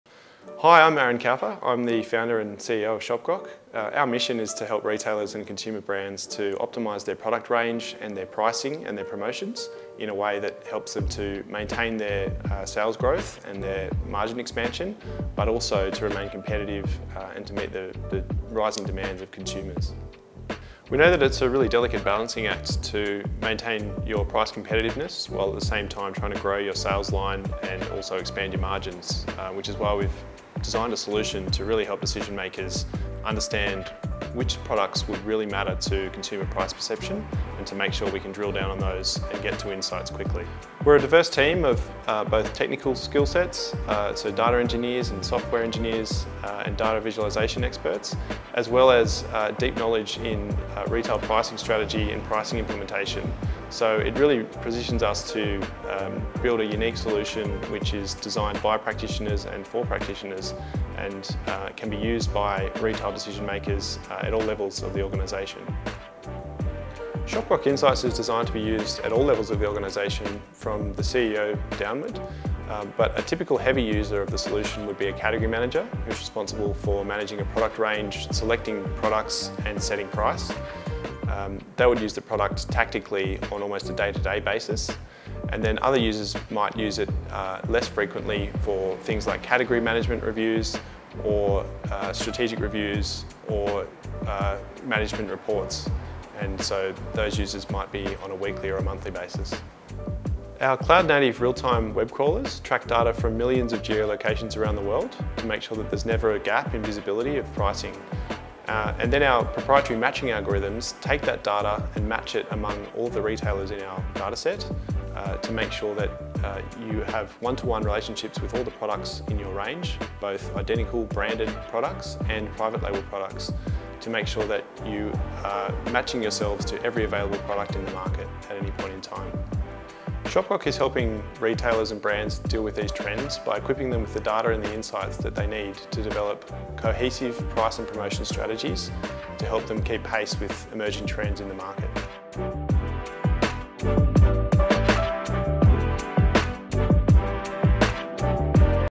Shopgrok - Intelligent pricing for big ecommerce
Shopgrok assists retail decision makers analyse their markets, choose range, set prices and plan promotions to drive profitable sales growth.

Modern retail moves at an extremely fast pace and decision making has to follow suit. Faced with a number of challenges, retailers must embrace advanced retail analytics in order to outperform the competition.